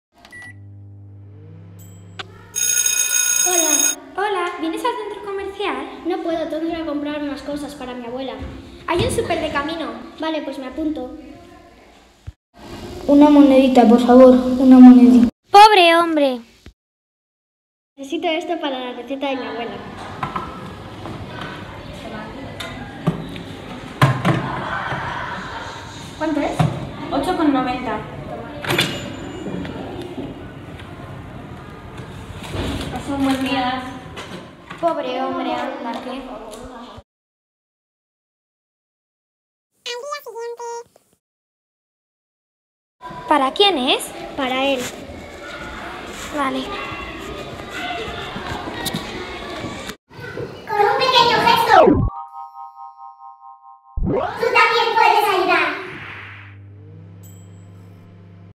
Con un pequeño gesto, tú también puedes ayudar
Diariamente, hacemos compras innecesarias, vemos a personas en situaciones difíciles y pasamos, hasta que un día nos sensibilizamos y compartimos lo que tenemos.